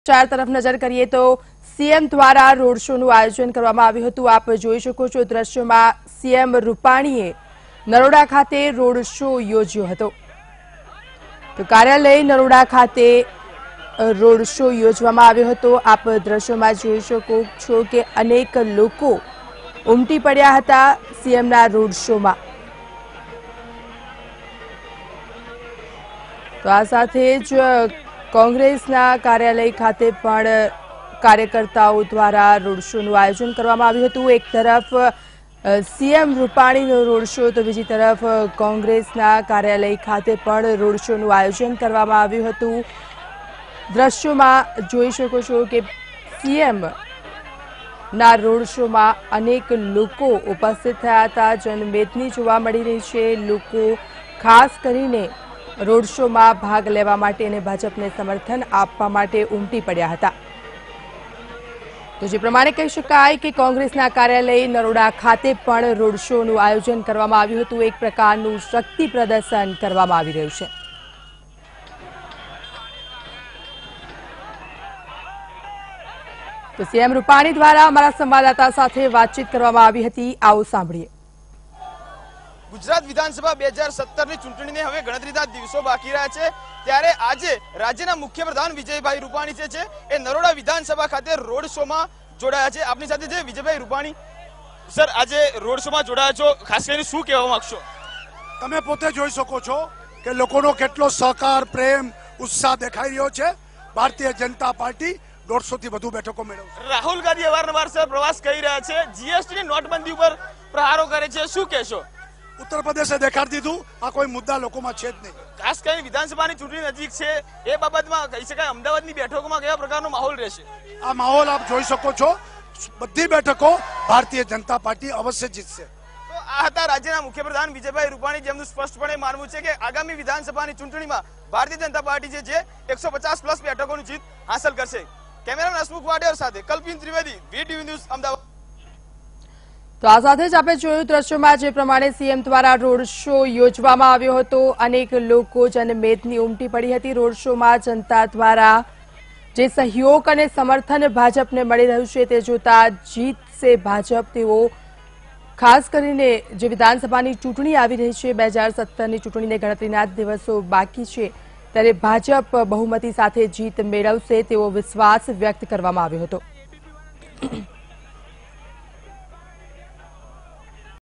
Gujarat Elections: CM Rupani attended BJP Road Show against Congress Office in Naroda | Vtv News
Gujarat Assembly Elections 2017: CM Rupani attended BJP Road Show against Congress Office in Naroda, Ahmedabad